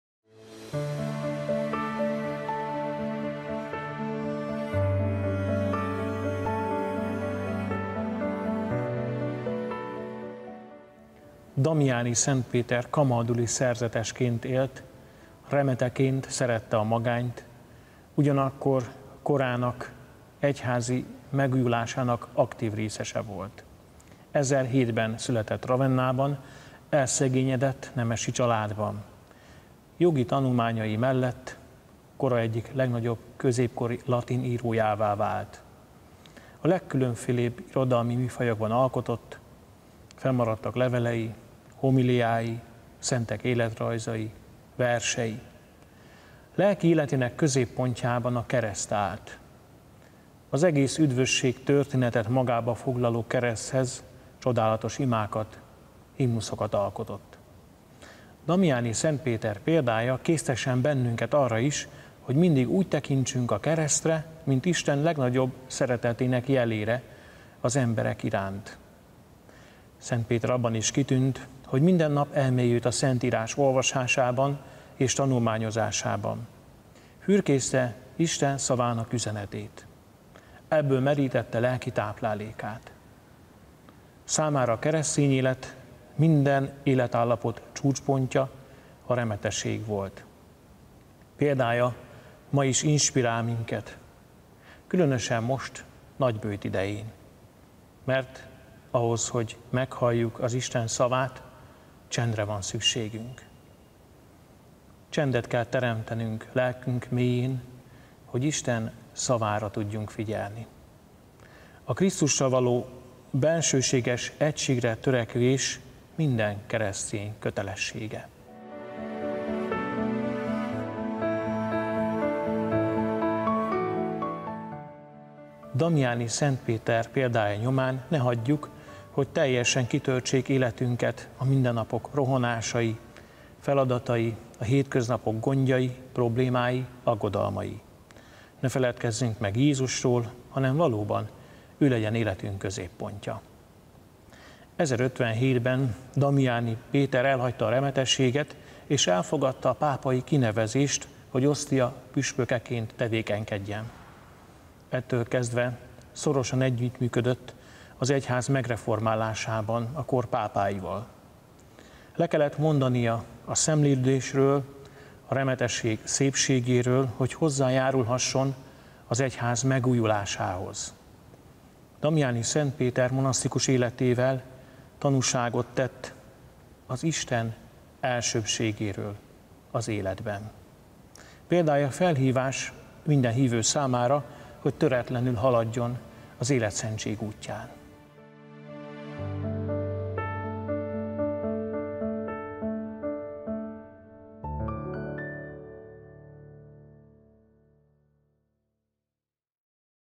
A HÉT SZENTJE – DAMIANI SZENT PÉTER
Damiani Szent Péter püspök és egyháztanító. A szegényeket, rászorulókat mindig segítette, magát keményen fegyelmezte. Bencés szerzetes lett, remeteségbe vonult, de IX. István pápa Ostiai püspökké nevezte ki.
Liturgikus emléknapja: február 21.
A hét szentje sorozatban Rolik Róbert atya elmélkedik róla.